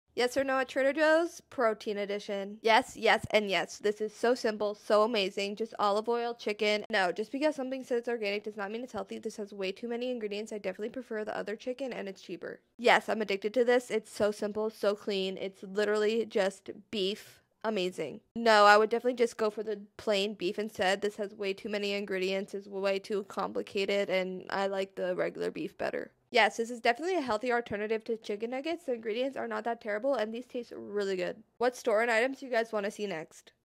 Yes or No at Trader Joe’s Protein Edition
yes or no at trader joes protein edition as a holistic health coach and nutritionist who’s realistic about ingredients but still cares. Not all foods have to be perfect I am just educating on what are better alternatives! Btw I’m a total protein enthusiast 😋 what stores should I do next?